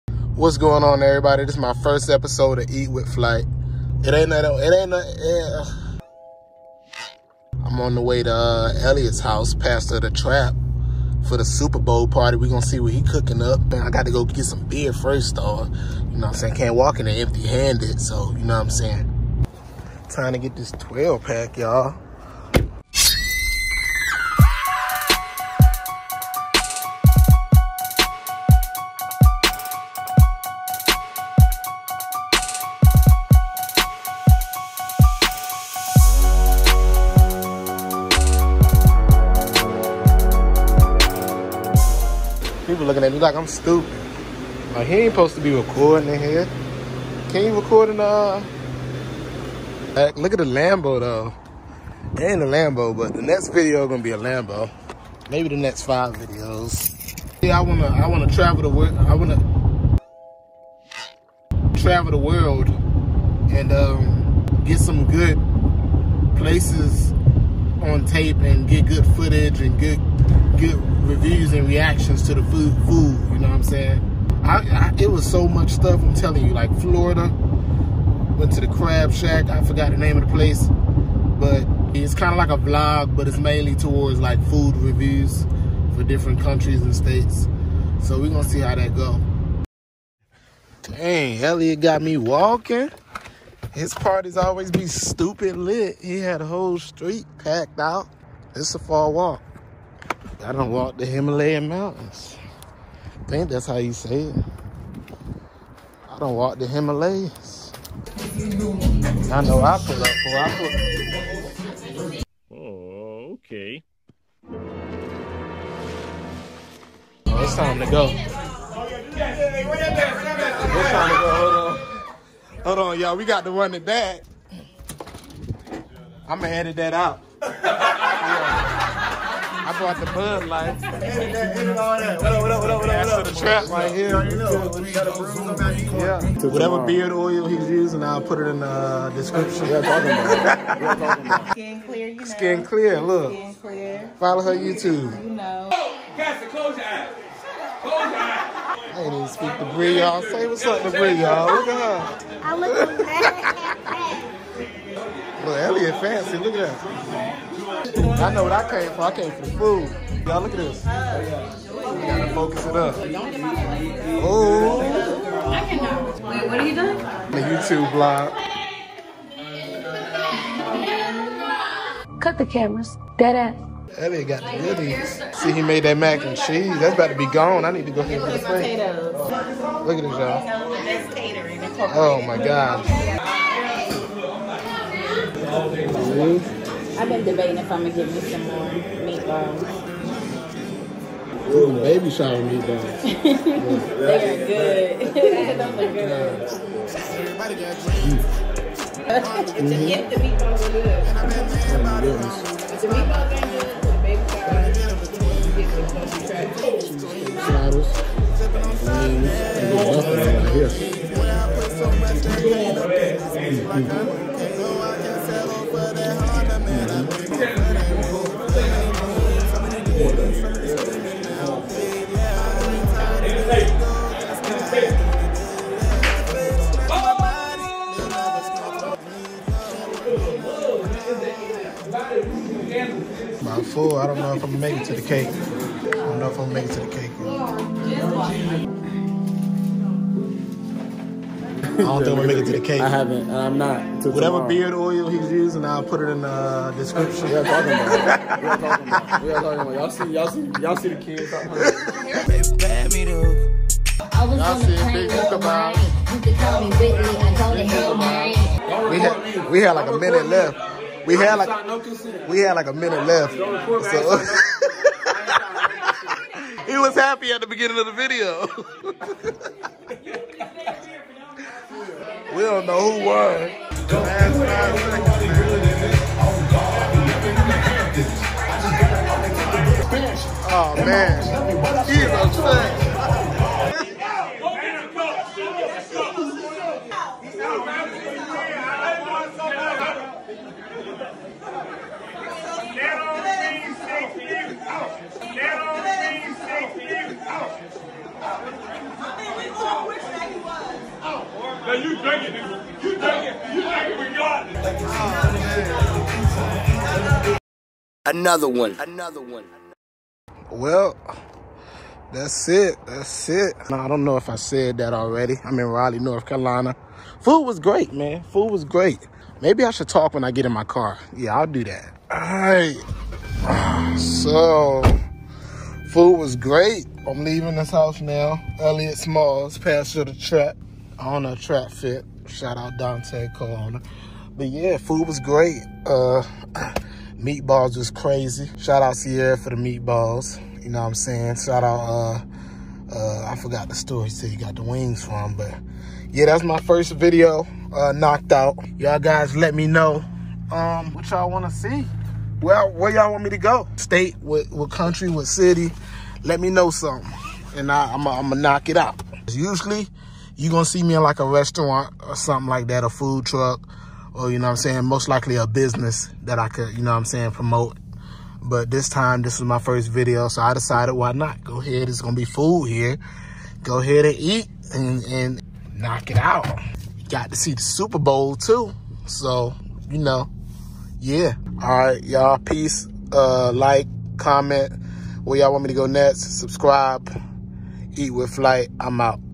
I Did A Food Review At Elliots' Superbowl Gathering || Things Got Turnt !
First Episode Of "Eat With Flight" Filmed At A SuperBowl Gathering In Raleigh NC